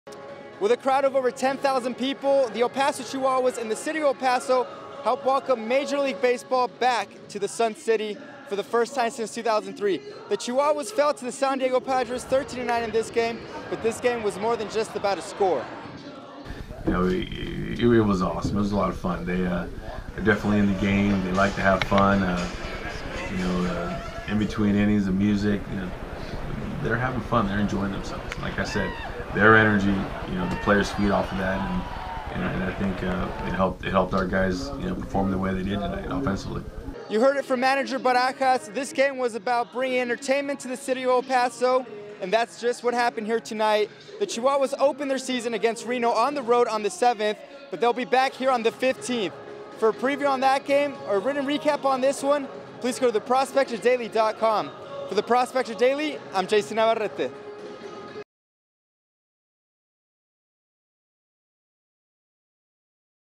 Chihuahuas Baseball
The El Paso Chihuahuas host the Sand Diego Padres.